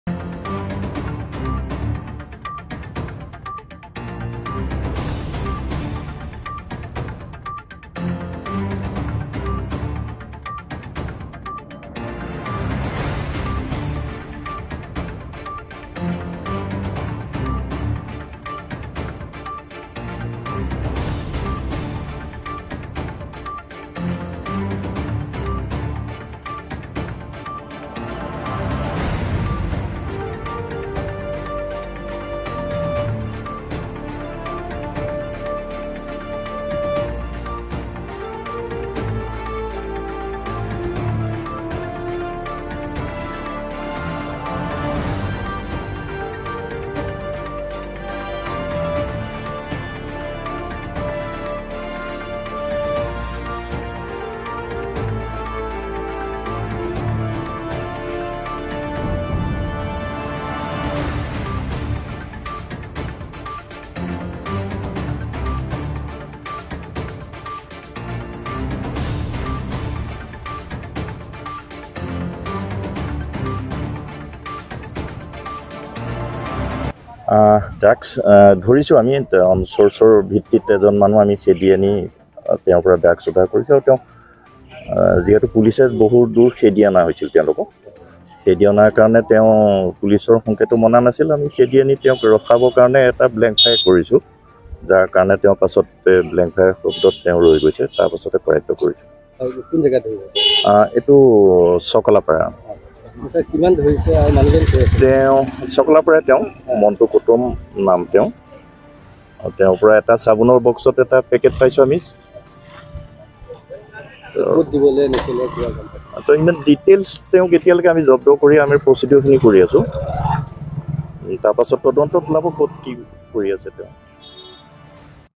গহপুৰত ড্ৰাগছ বেপাৰীক আটক
ৰাজগড় আৰক্ষীয়ে ৰাজগড়ৰ পৰা খেদি আনি ১নং চকলাত কৰায়ত্ত কৰে ড্ৰাগছ ব্যৱসায়ী  মন্টু কুটুম(৪৪)ক ।পলায়ণ কৰা মন্টুক কৰায়ত্ত কৰিবলৈ আৰক্ষীয়ে এজাই শূণ্যত গুলী চলাই।লগে লগে বাইক এৰি পানীত জাপ দিয়ে মন্টু কুটুমে। পানীত সাতুৰি গৈ মন্টুক ধৰিবলৈ সক্ষম হয় আৰক্ষী। অসম-অৰুণাচল সীমান্তৰ ৰাজগড়ত কোনো ব্যক্তিক দিবলৈ আনিছিল ড্ৰাগছখিনি। আৰক্ষীয়ে মন্টুৰ পৰা ১২ .৫৩ গ্ৰাম ড্ৰাগছ জব্দ কৰাৰ লগতে এখন  AR 01E4314 নম্বৰৰ ডিছকভাৰ বাইক জব্দ কৰে। ড্ৰাগছৰ আনুমানিক মূল্য ৪০,০০০ টকা।